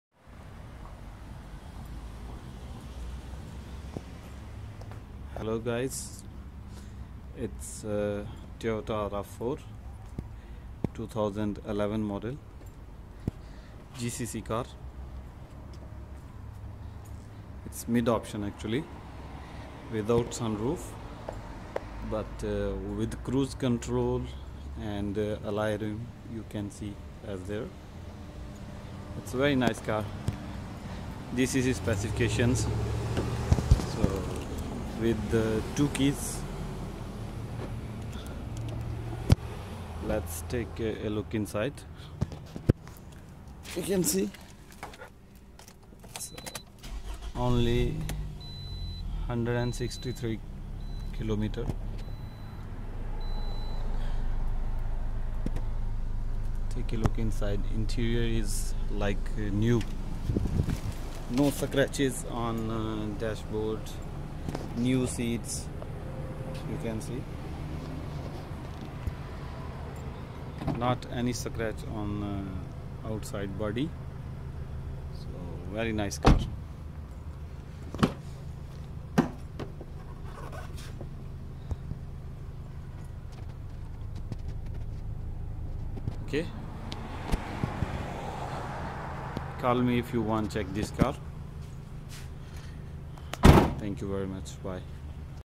Toyota RAV4. , 2011 model , GCc specific ,,
It's GCc rav4.  , 2011. Model only 160000 km , available in sharjah UAE ,
souq Al haraj , shop no 216